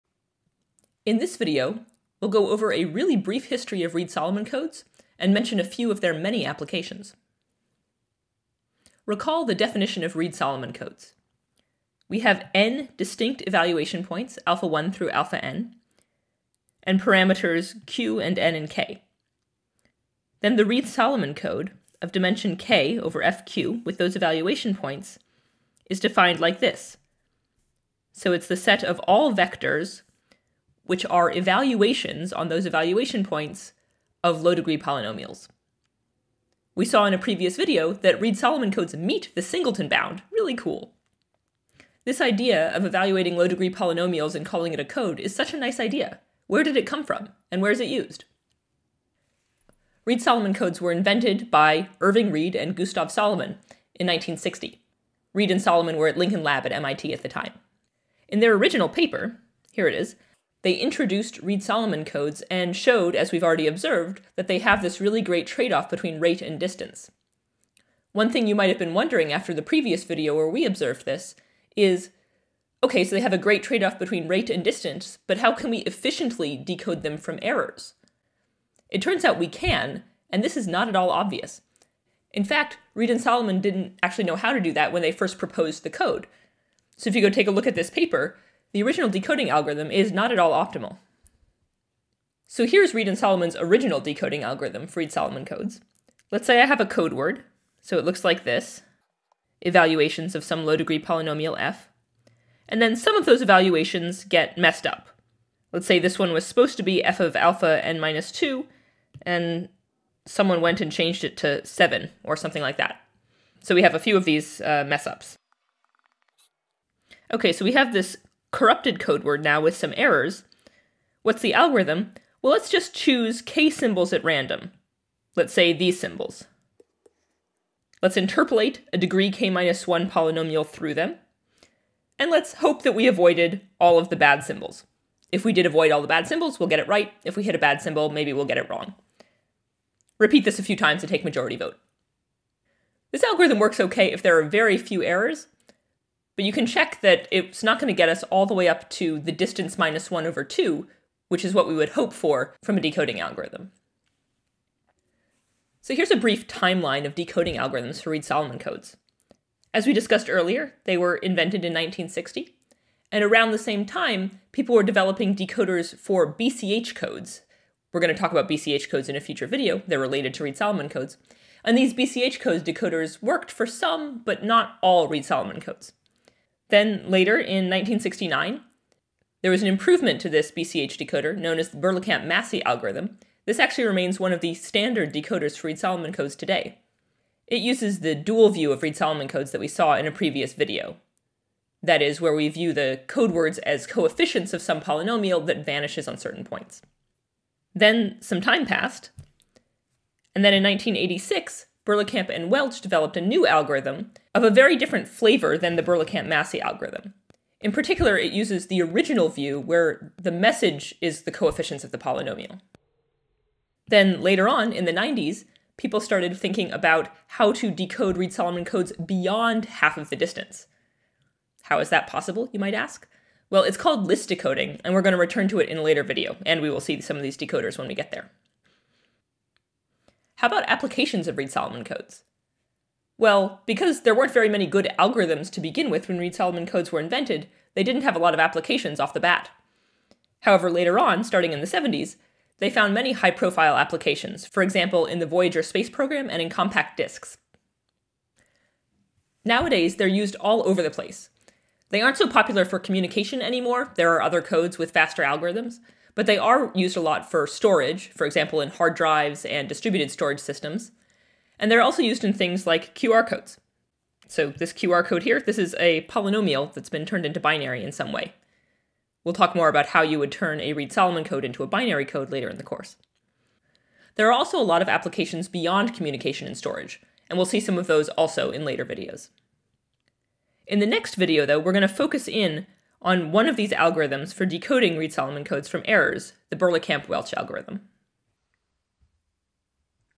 Lecture 5, Video 1: History of RS Codes
A (very) brief history of RS codes, their decoding algorithms, and applications.